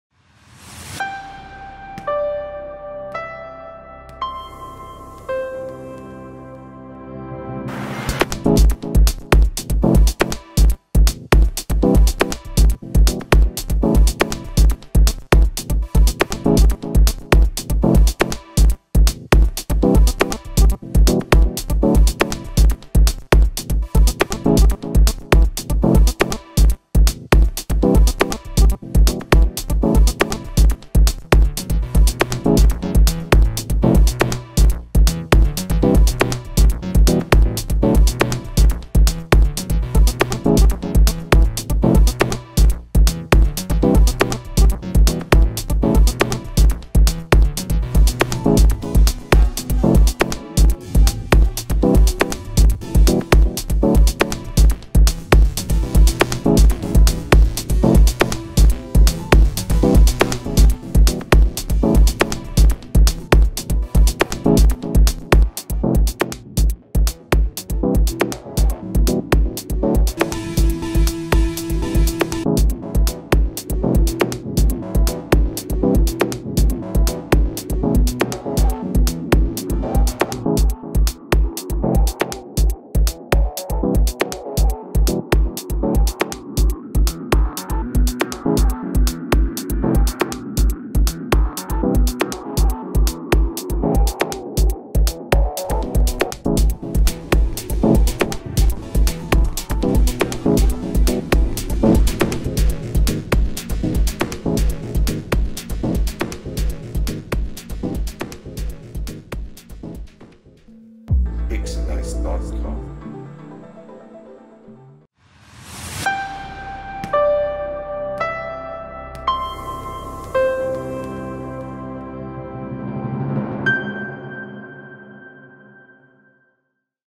making of neues Parkhaus im Wirtschaftspark IZ NÖ-Süd # 1
Kurzvideo zur Entstehung unseres neuen ecoplus Parkhauses im IZ NÖ-Süd. Nach diesem ersten Video folgen bis zur Inbetriebnahme im Jänner 2019 einige weitere..